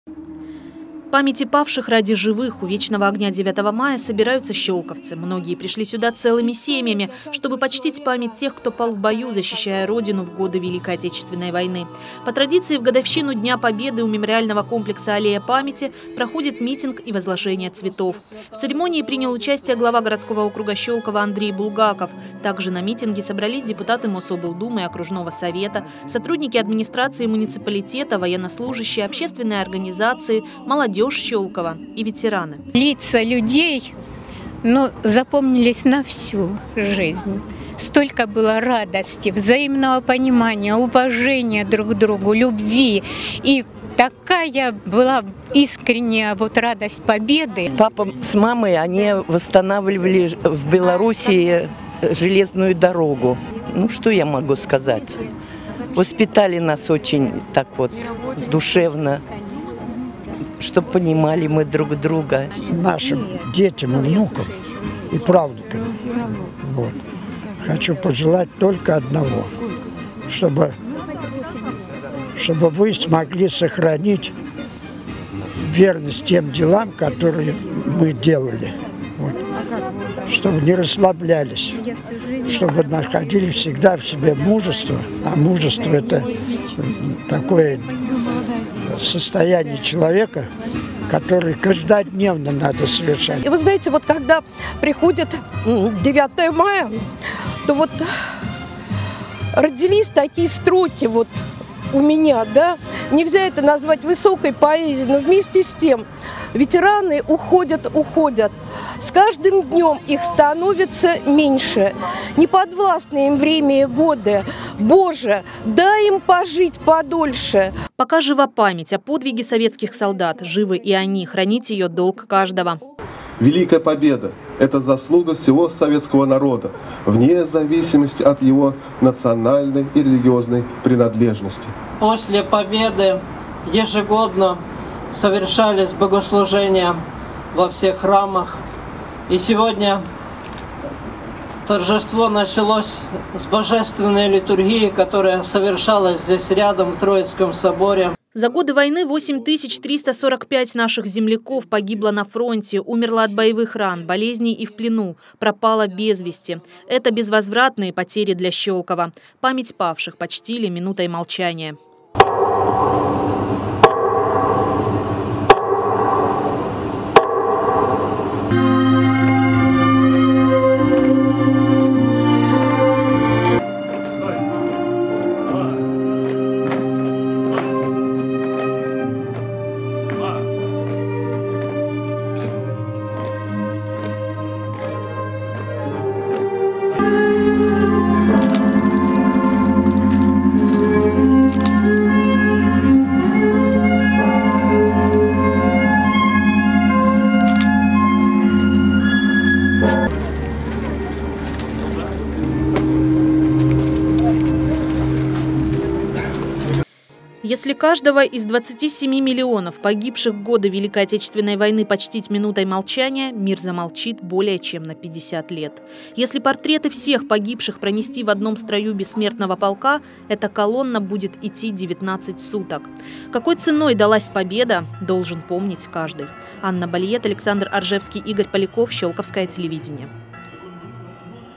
Глава округа Андрей Булгаков принял участие в праздничном мероприятии на Аллее Памяти
9 мая у мемориального комплекса "Аллея Памяти" состоялись митинг и возложение цветов. В церемонии принял участие глава округа Андрей Булгаков.